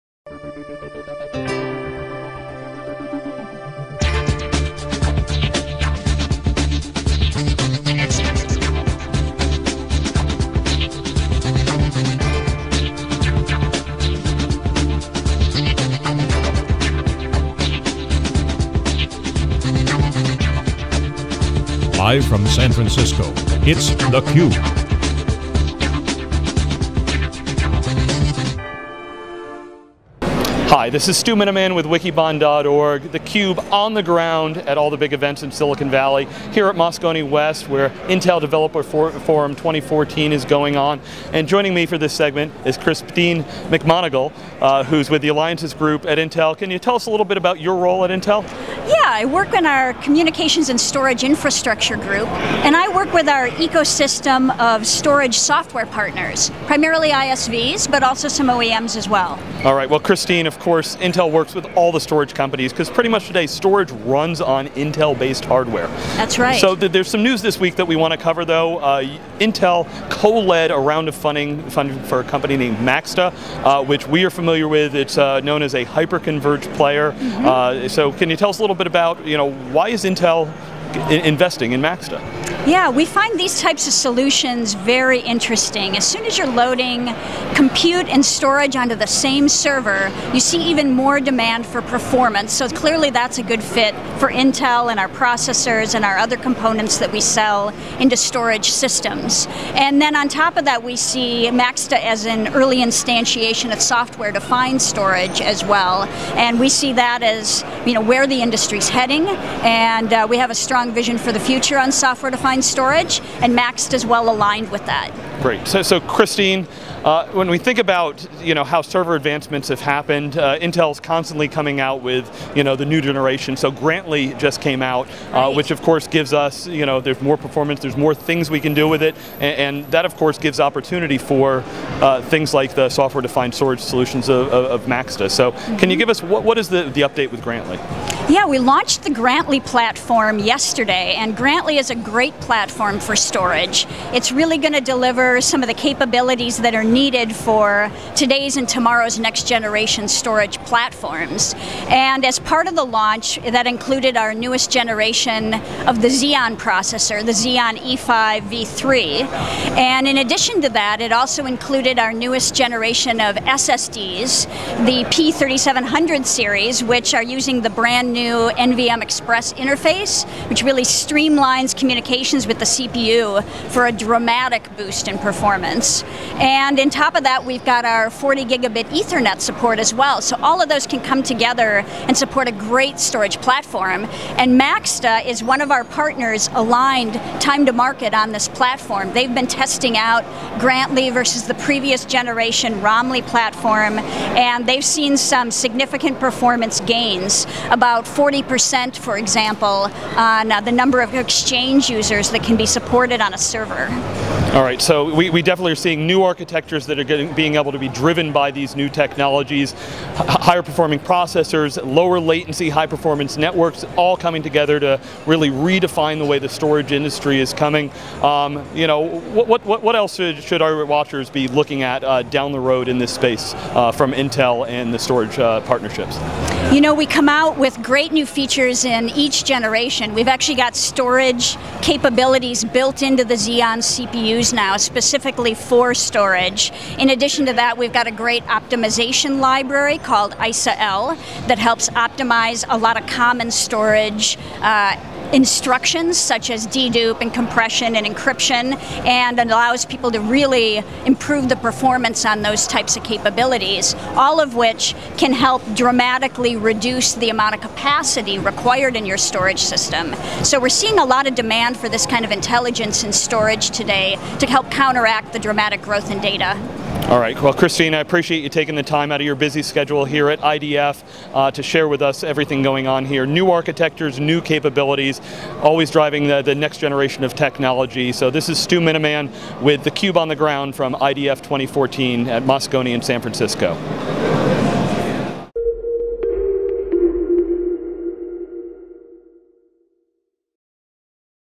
Christine McMonigal - Intel Developer Forum 2014 - theCUBE
Christine McMonigal, Intel, at Intel Developer Forum 2014 with Stu Miniman

With up to 18 cores and an array of data-driven software capabilities, the Xeon E5-2600 v3 marks one of the biggest leaps in Intel Corp.’s flagship server processor series yet. The new iteration raises the competitive bar beyond merely keeping up with Moore’s Law to addressing specific workload requirements down at the component level.

The E5-2600 v3, which made its debut at the Intel Developer Forum,  comes in 26 separate models designed to address an especially broad spectrum of use cases ranging from office computing to distribute data crunching. Added to the list are another 20 custom-ordered configurations that Intel claims to have already started shipping to select customers, whom it didn’t name but presumably include many familiar names from the hyperscale market, which has a known appetite for what Wikibon terms hyper-specialized hardware.

The new family represents Intel’s most serious attempt thus far at bringing some of that optimization down into the traditional enterprise data center.  The most expensive E5-2600 v3 chips can support up to 70 percent more virtual machines  than their predecessors, according to the company, which can add up to a lot of saved licensing costs in large organizations. Since hypervisor vendors such as VMware Inc. charge per core, the the denser the infrastructure, the less money a customer has to pay for software.

To top it off, the E5-2600 v3 introduces a built-in voltage regulator that allows each individual processor in a chip to set its own power level depending on the load, a feature that can also produce significant cost savings in large-scale environments. The family further cuts  electricity consumption through the use of DDR4 memory, which succeeds the DDR3 architecture used in the previous generation  series and provides increased data reliability with 35 percent greater efficiency.

The fourth-generation specification can also handle considerably more bandwidth, an improvement that Intel is exploiting with a new control that supports 10 Gigabit Ethernet and – continuing the power optimized theme – uses half the energy of its predecessor. Yet while the increased efficiency is a tremendous advantage in and of itself, Intel says that customers will not only be paying less for software licenses and electricity with the E5-2600 v3 but also get more back.

Every chip in the family comes fitted with thermal sensors for monitoring airflow and outlet temperature, a potentially powerful maintenance tool in the hands of operations professionals, and a brand new supporting chipset that can support three USB 3.0, eight USB 2.0 and 10 SATA3 slots. Also included in the package is compatibility with the NVMe protocol for connecting direct-attached flash cards.

Overall, the  E5-2600 v3 performs up to three time faster than the previous generation Xeon series and can handle six times as much throughput at half the latency. Combined with the value-added capabilities Intel has baked into the series, that makes for a powerful value proposition that puts the chipmaker in a that much better position to extend its dominance into the software-defined data center.